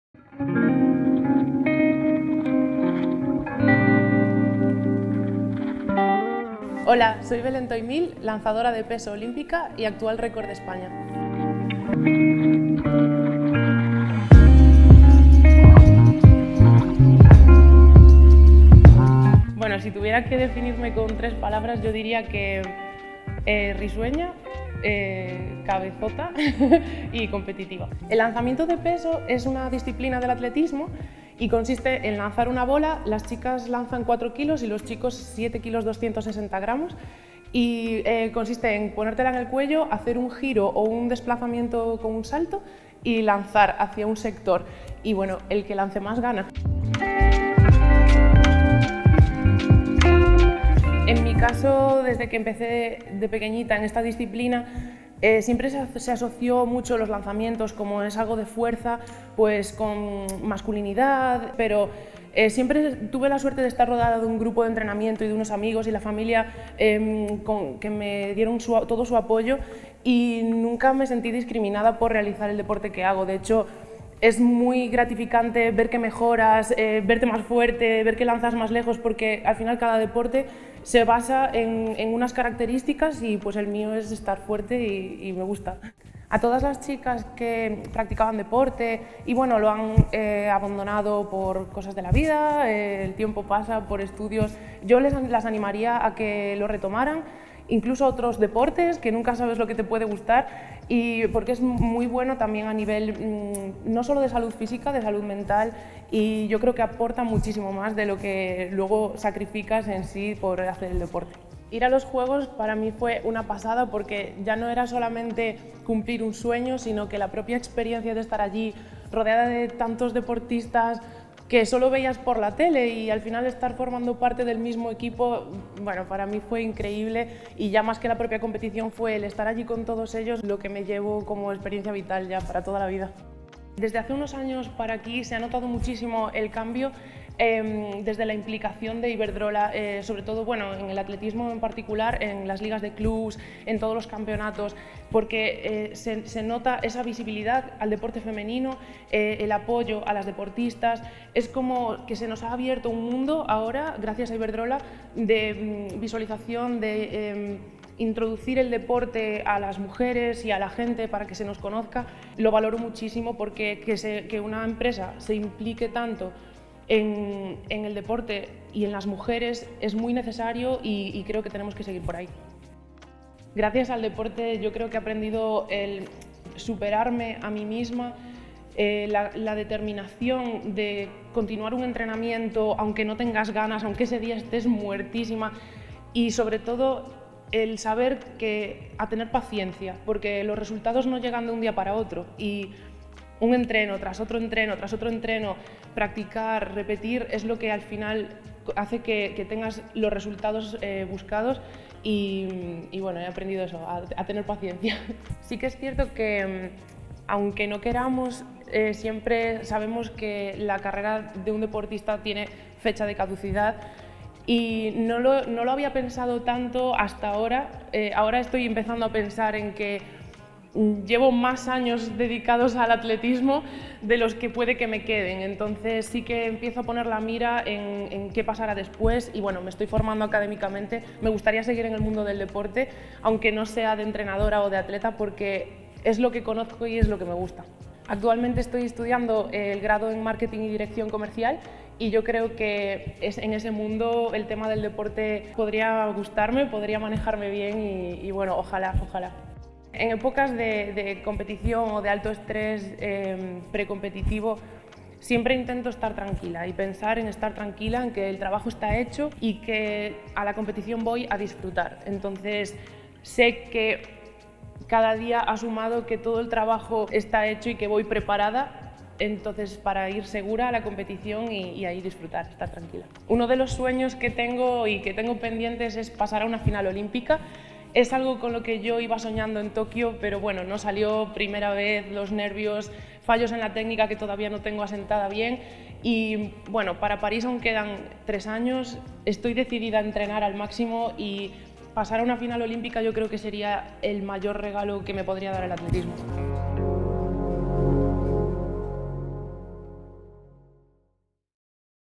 Belén Toimil: Muy fuerte
Risueña, cabezota, competitiva y con una sensibilidad cautivadora, Belén Toimil Fernández (Mugardos, La Coruña) es una auténtica cariátide. Su fortaleza (física, mental y emocional) sí que es 360º y gracias a ella sostiene una determinación que bate récords y rompe estereotipos. Descubrimos su historia en un nuevo capítulo de Mujeres en el deporte. Un proyecto de TELVA para IBERDROLA.